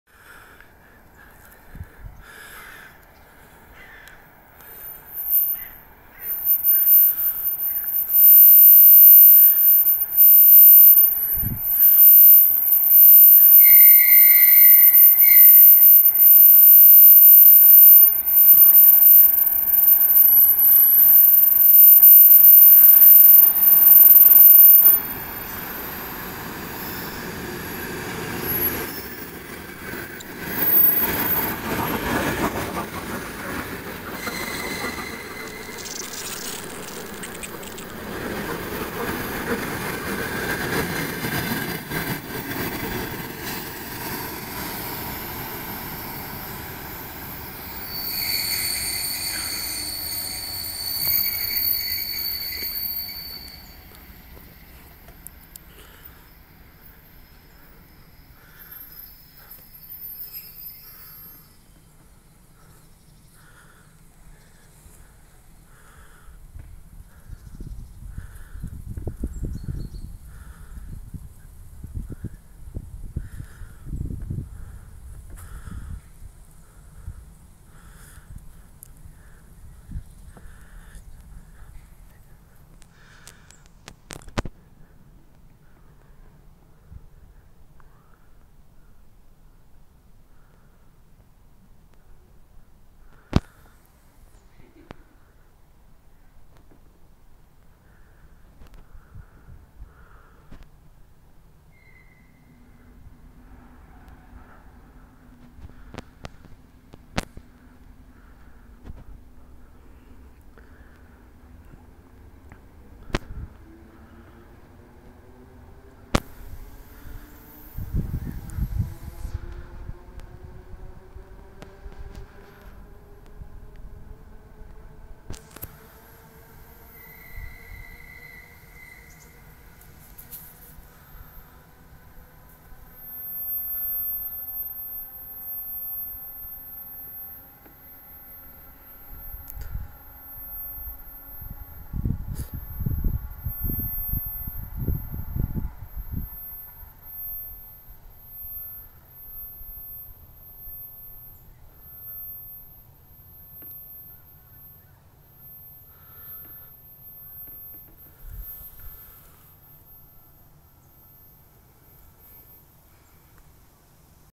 EMU train ER2-8029 on the wrong track at station Jaundubulti
ER2-8029 (Tukums-1-Rīga)
Jaundubulti, Jūrmala, Latvia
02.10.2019
14:52